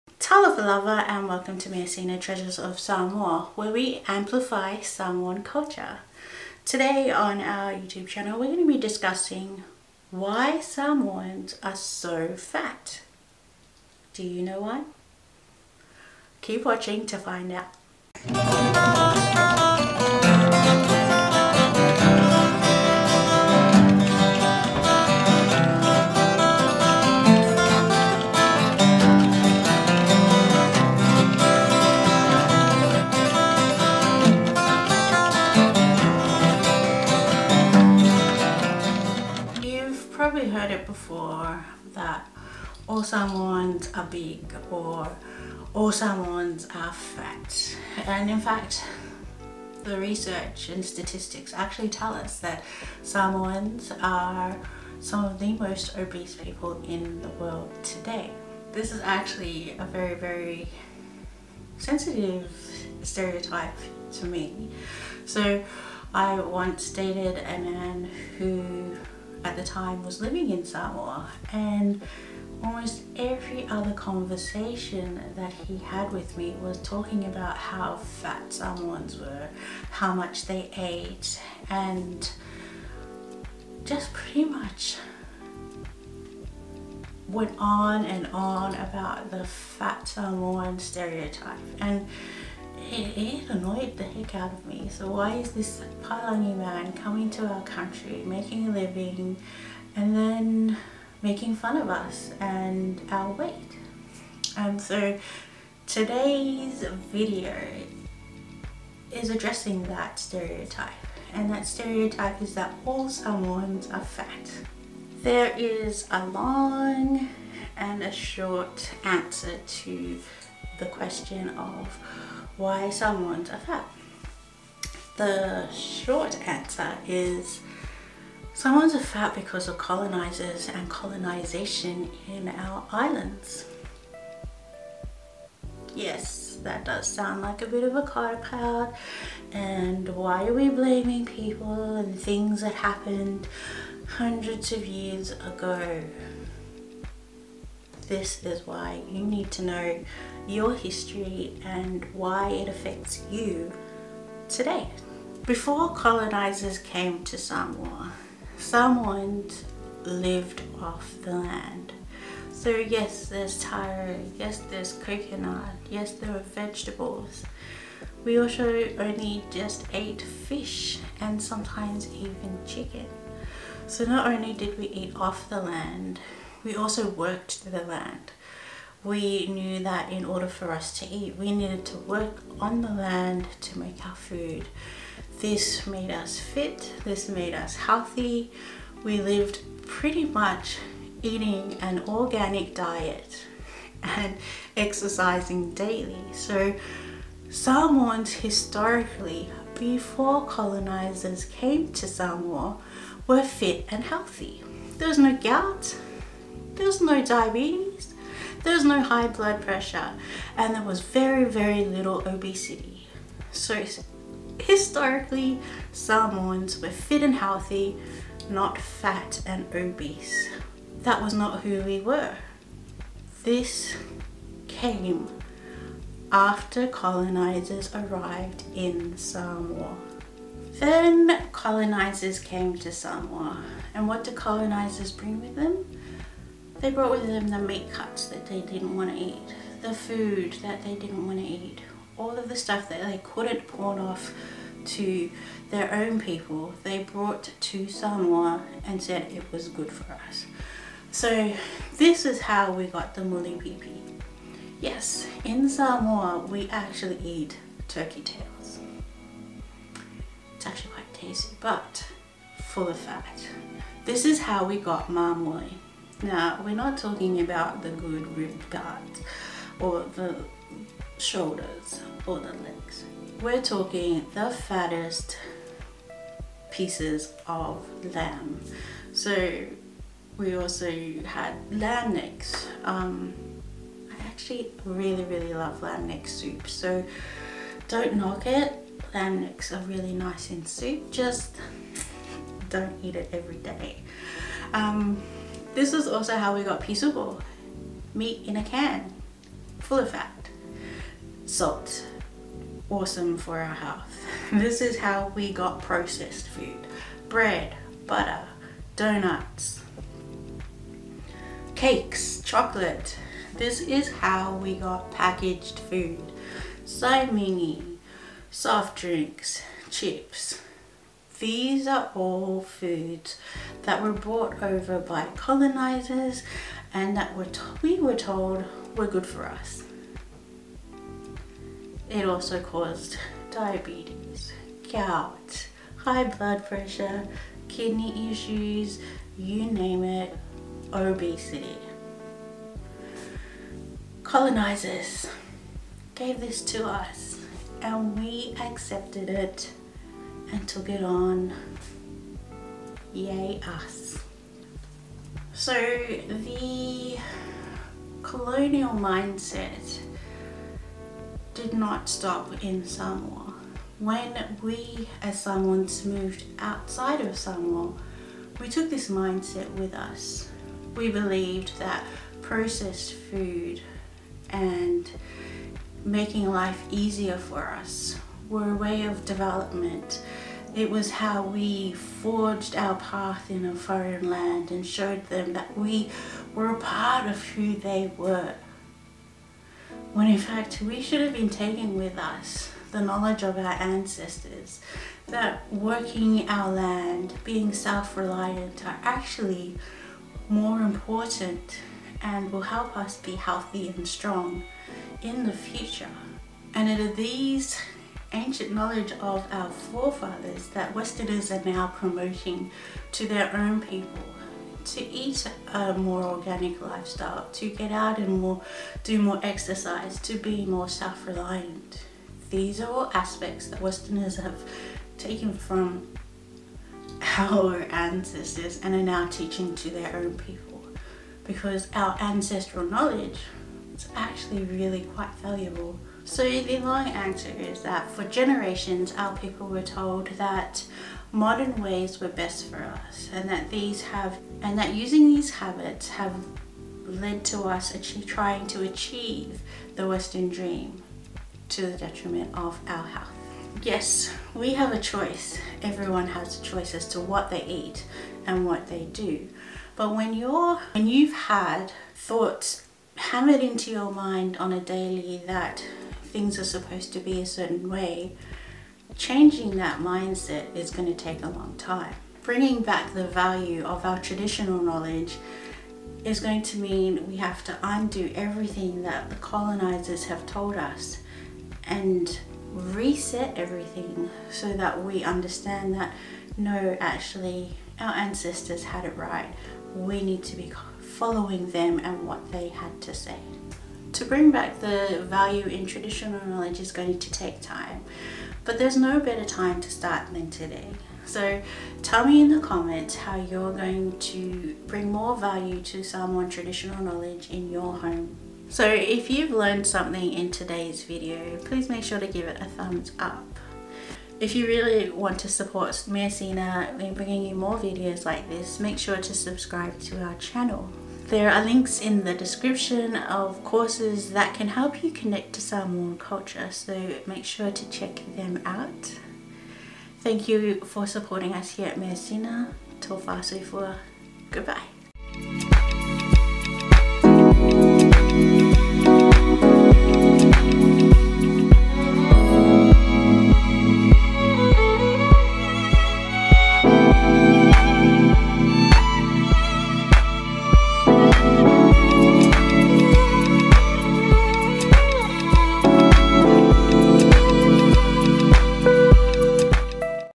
WHY ARE SAMOANS SO FAT?
Samoans are known for being big and strong, we are also some of the most obese people in the world.  This video discusses the causes of why Samoans are so fat.

Teach your child Samoan language and culture at home with Tamaiti